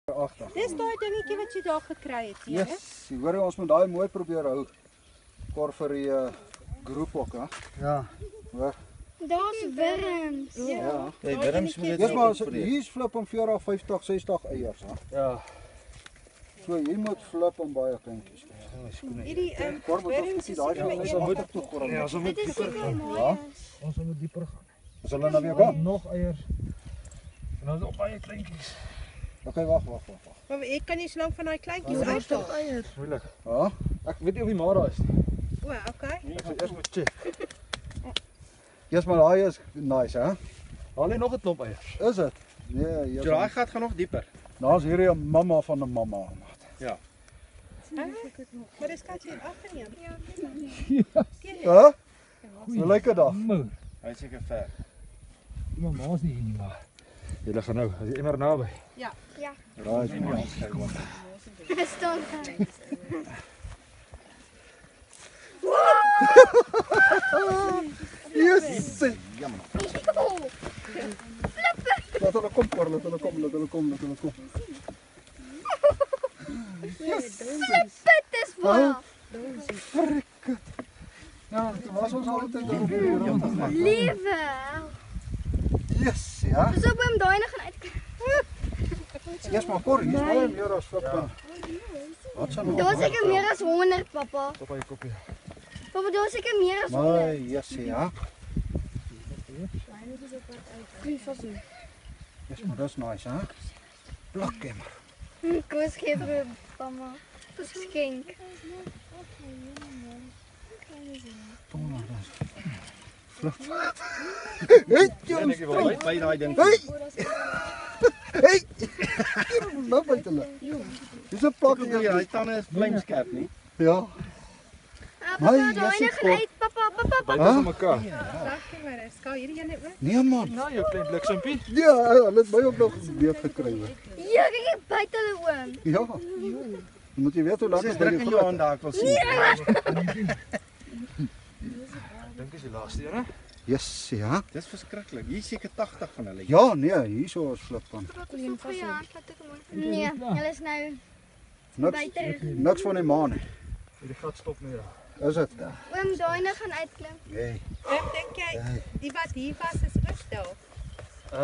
68 Southern African Python babies extraction
These 68 baby Southern African Pythons were relocated because the farm workers would've killed them so I took them to a nature reserve where they will be safe from ignorant people😊